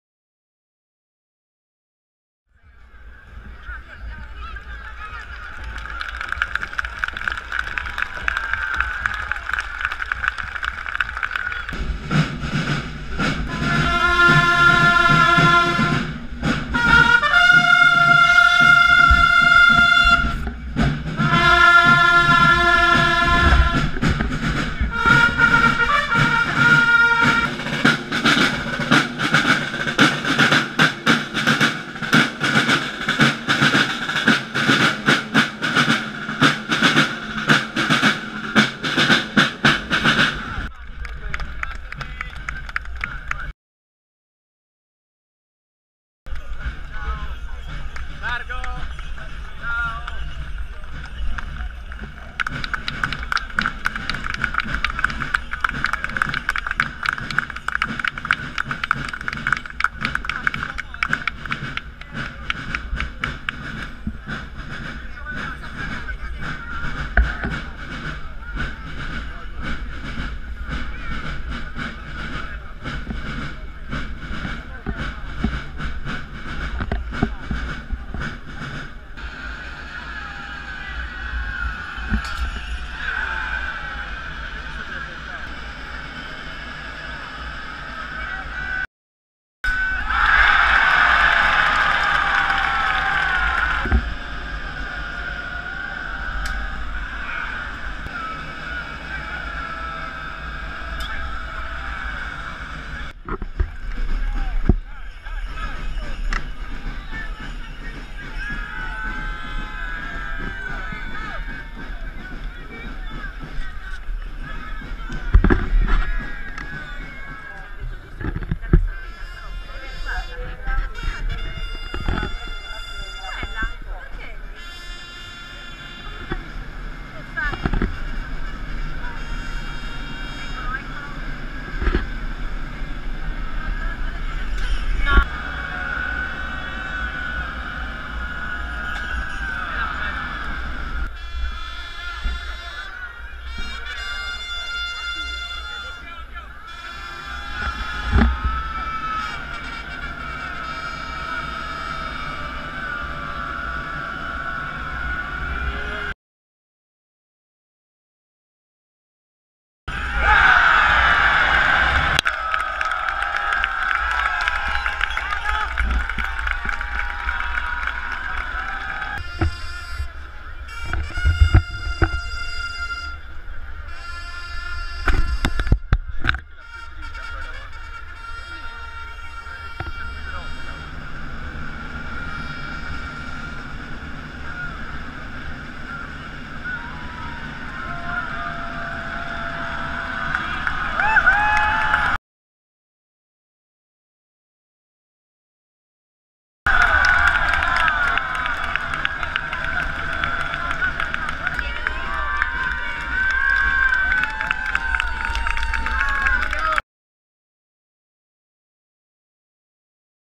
Momenti di Sartiglia 2014
Momenti di Sartiglia, domenica, vissuti con la mia GoPro.